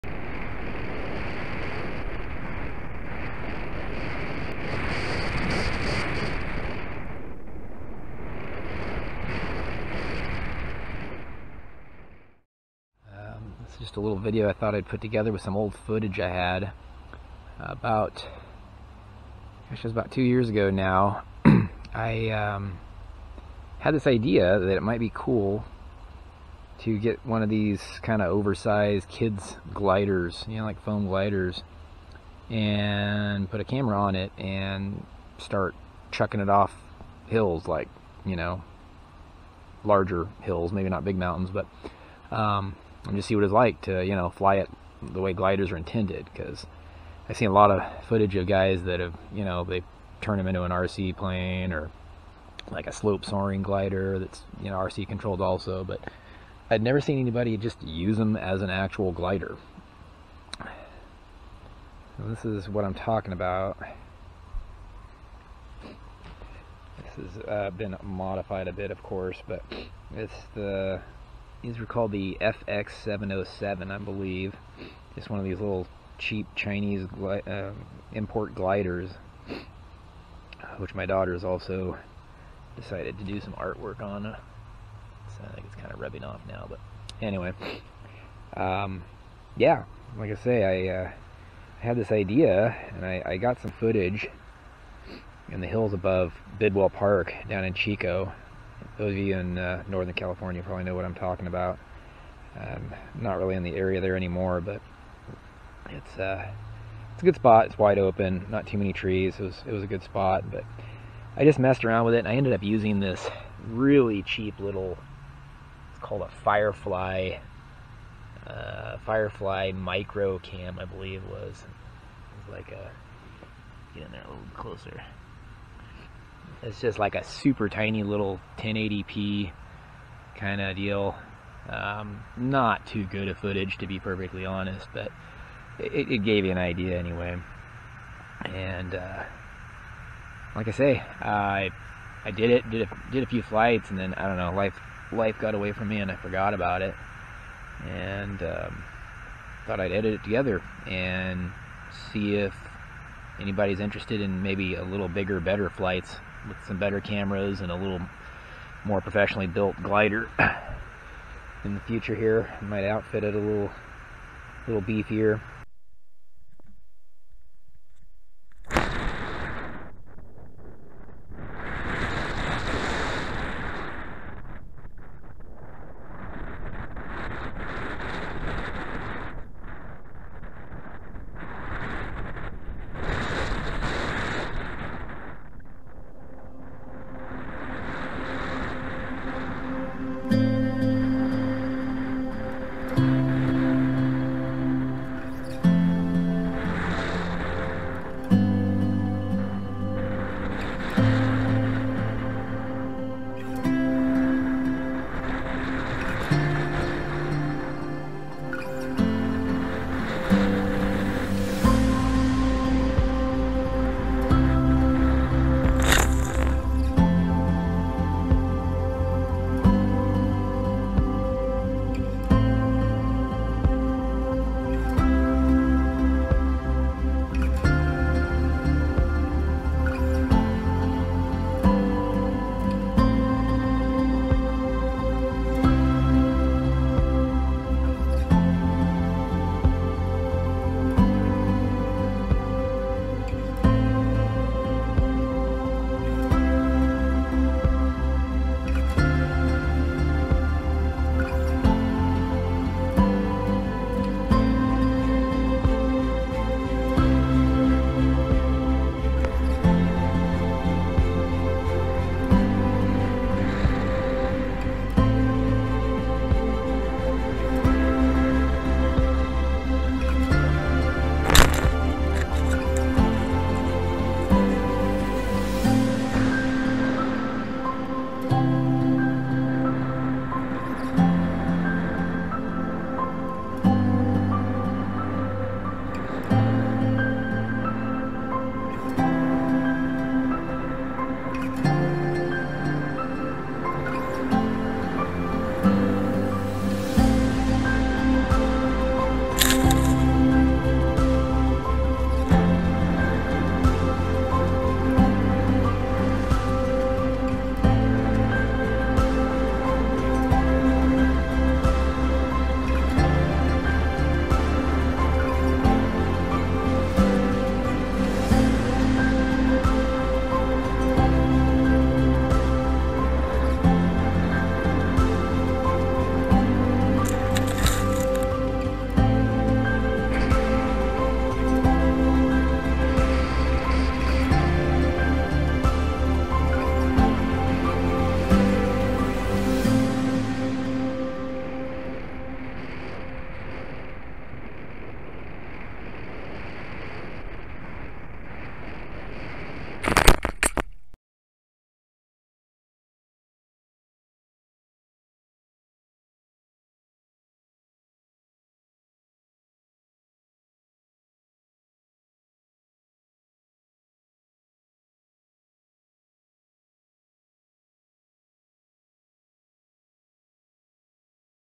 Tossing a foam glider off a hill....how far will it go ??
Just some FPV footage from a 4 foot wingspan glider flying off the hills outside Chico CA completely uncontrolled, my plan is to outfit it with a better lighter 4k camera and GPS tracker and go for some much larger mountains and long range flights.   These things fly great and far but its harder than it looks to get balanced just right for this kind of flying as they always want to curve back towards the mountain after a certain distance.

The plane used is really hard to find now and totally overpriced compared to what i paid but heres a

The Camera was a Hawkeye Firefly Micro Cam, they dont exist anymore best as I can tell and frankly don't have a very good picture anyway, I only used it because it was super light and compact, thankfully there are a number of good tiny 4k options now that I plan to start using.